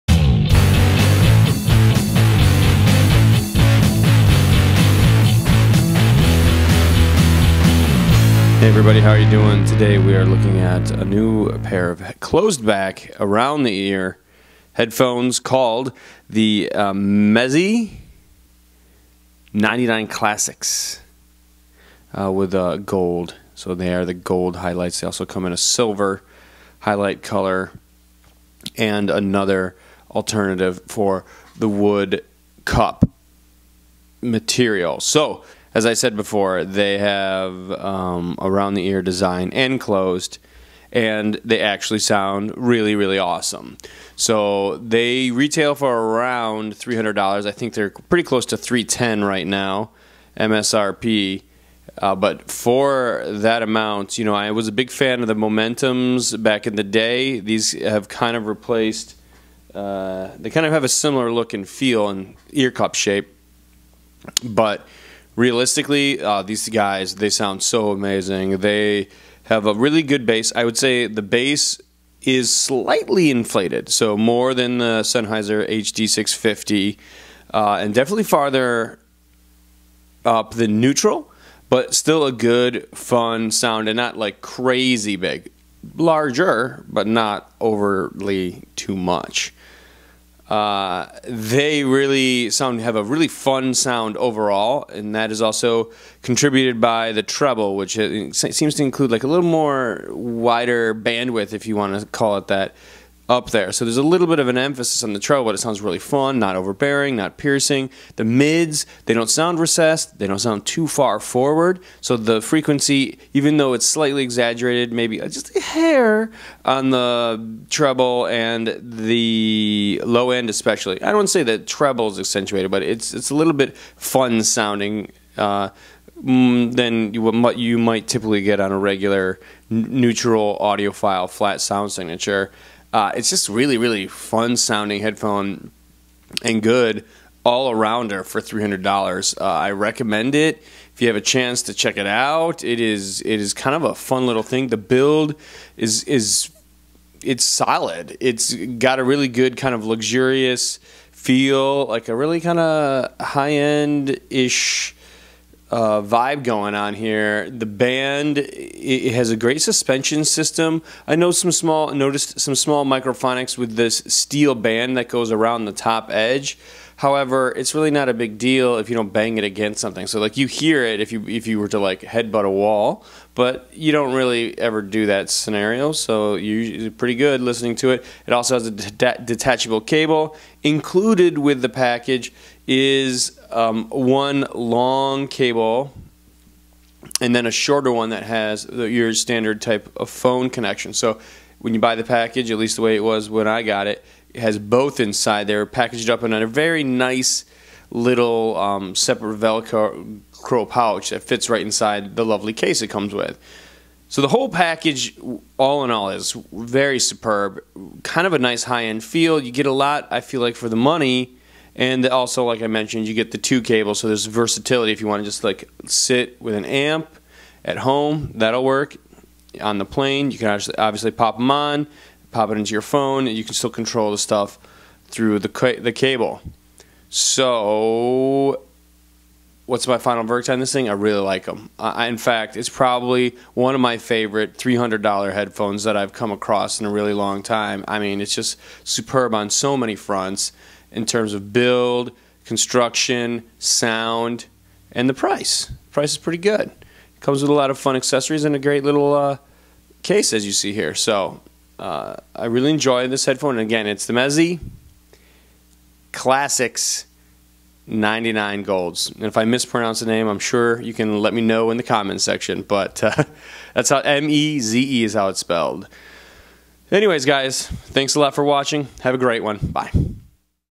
Meze 99 Gold Classics Headphones
The is a video review of the new MEZE 99 Classics with gold highlights. MSRP is $310 US.